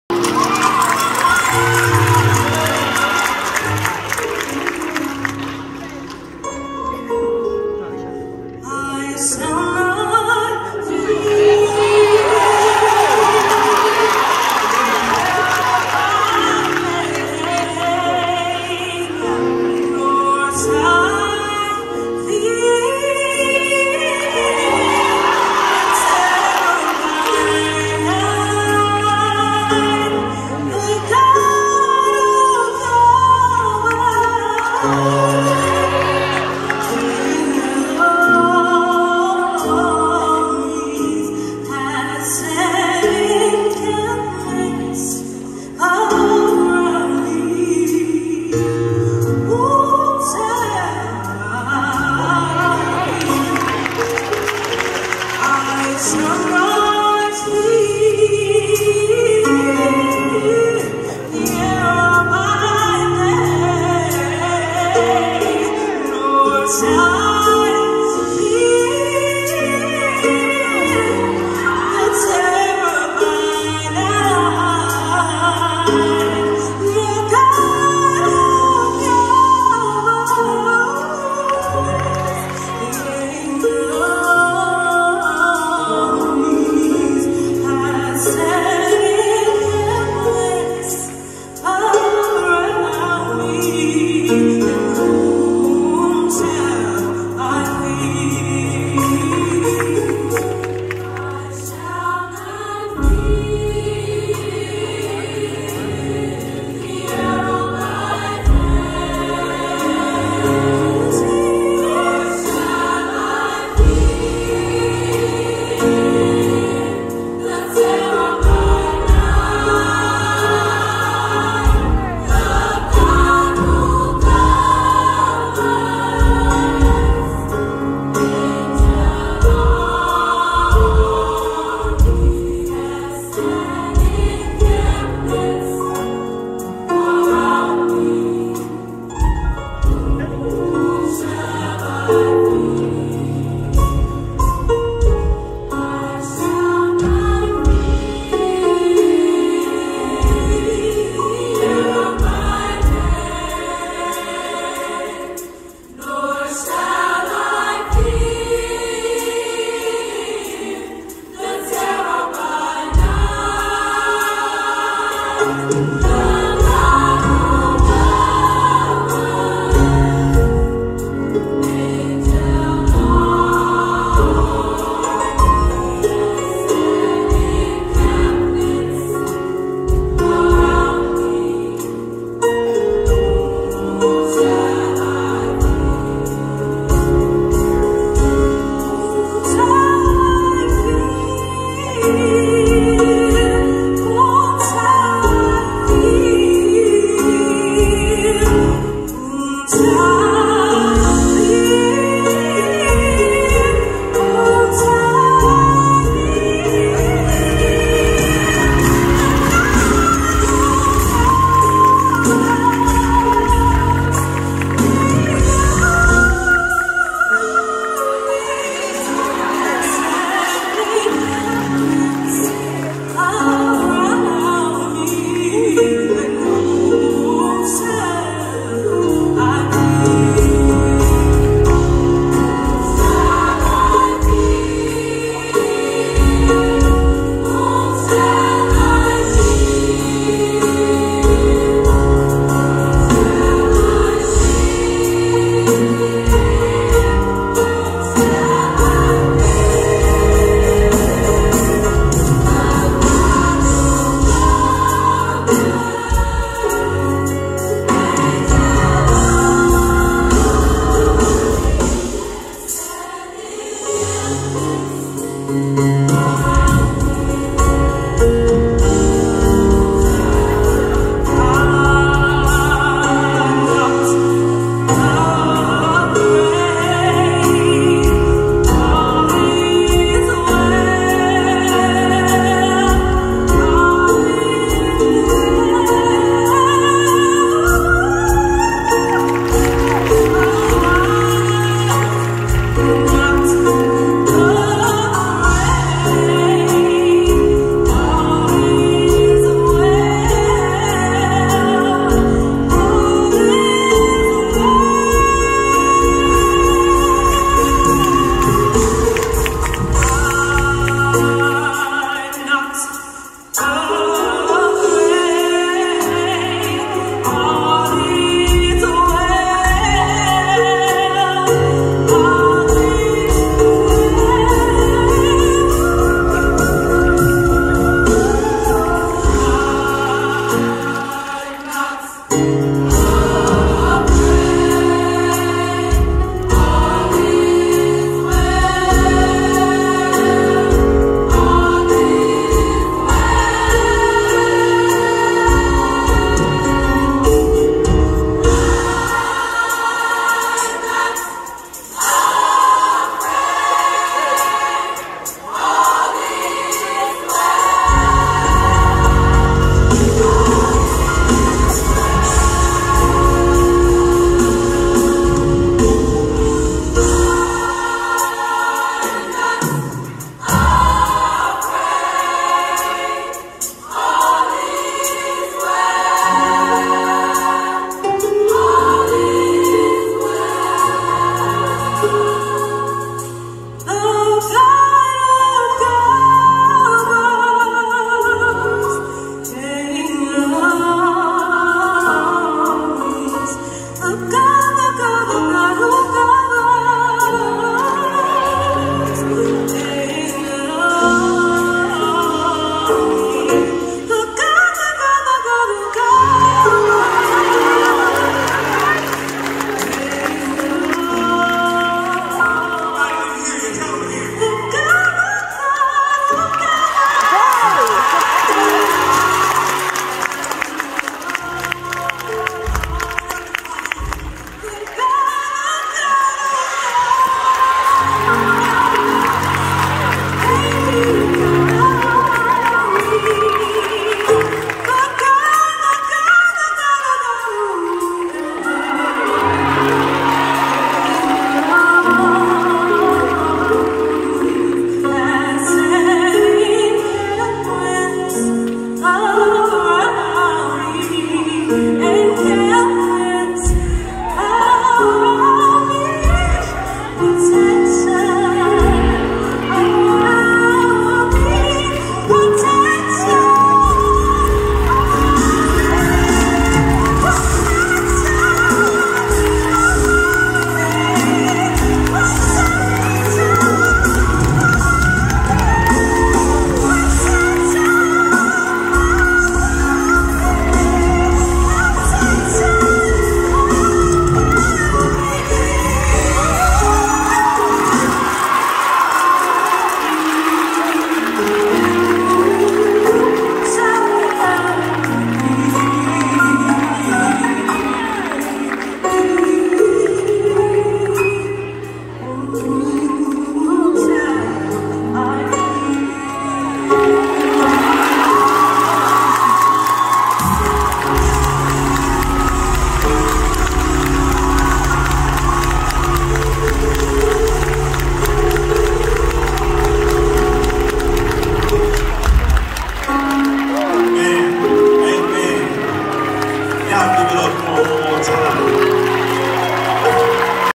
Carver High School choral choir
Performing at Joy night  at True Divine Baptist church.
Director  Mr.Henry Terry